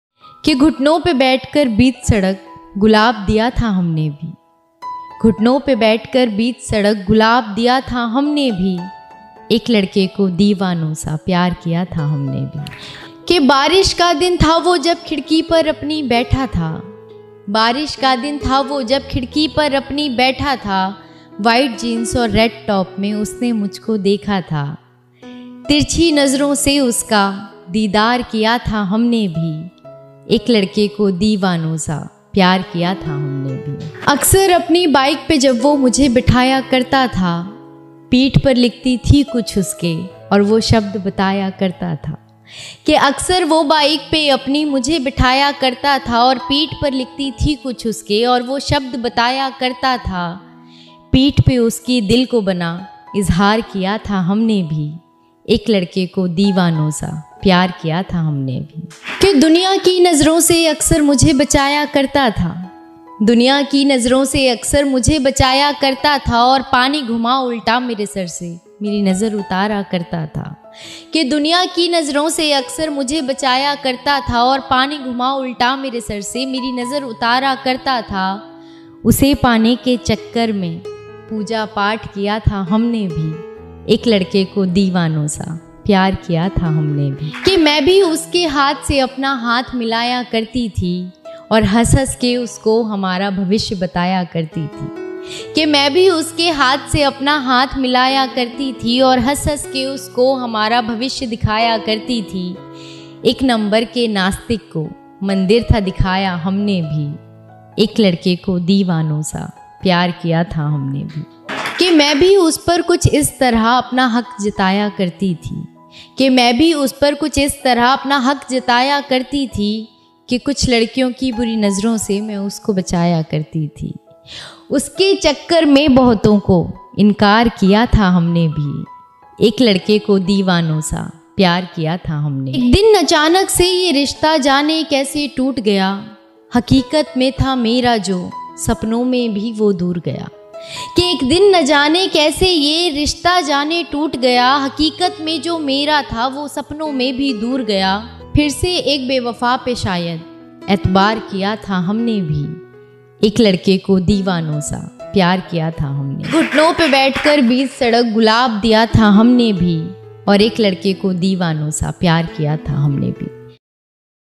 एक लड़के को दीवानों सा प्यार कियाथा हमने भीgoonjwaves
ghutno par baith kr bich sadak par gulab Diya tha hamne bhi
hindi poetry
white jeans or red top me usne dekha mujhko tha
pith par uske dil banake ejhar Kiya tha hamne bhi
uske chakkar me bahuto ko inkar Kiya tha hamne bhi
hindi poetry
the social house
#rahat ki sayari

Details